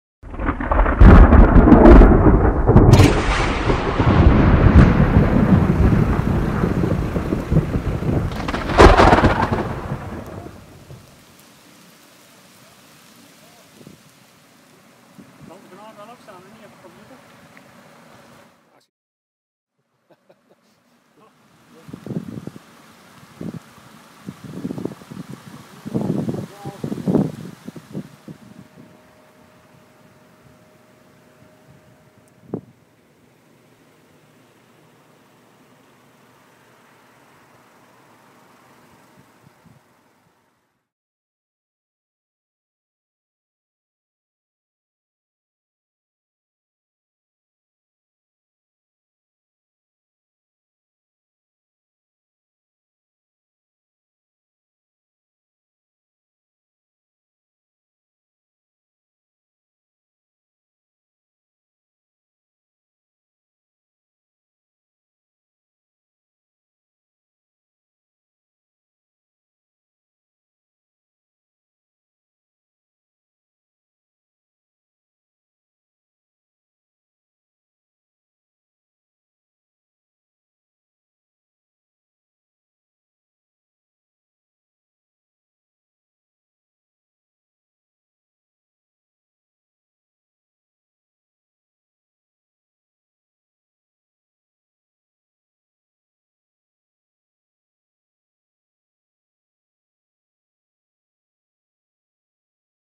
Bliksem splijt boom in Noordijk
NEEDE/NOORDIJK - Bliksem splijt een boom aan de Elsmansdijk in Noordijk. Door de blikseminslag 'explodeerde' de boom en vlogen stukken van de stam in het rond. De gespleten boom is uit veiligheidsoverwegingen door de gemeente Berkelland omgezaagd. Getuigen zagen een bolbliksem. Video (HD) Jelle Boesveld (iphone-4)